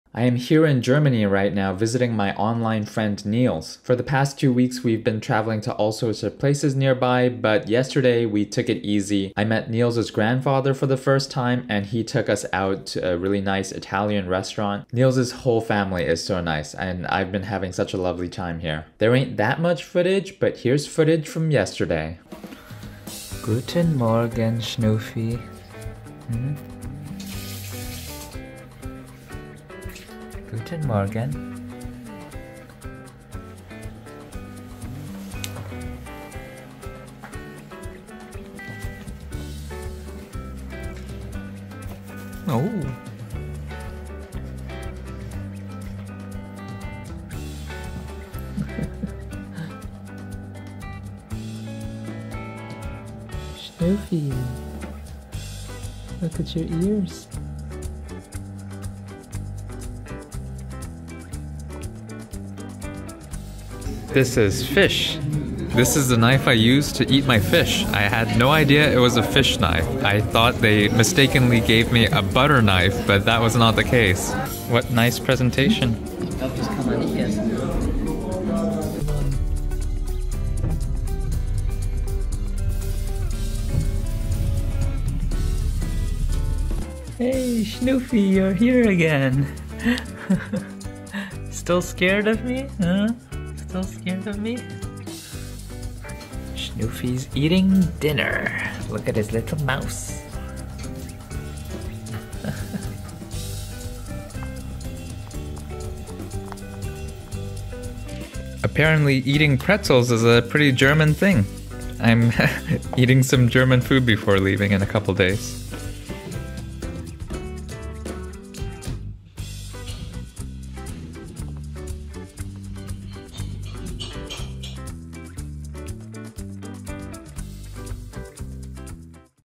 Chill day in Germany with my online friend - 1% Better Day 159
Took a nap because of jetlag. Time to go back in a couple of days and have jetlag all over again.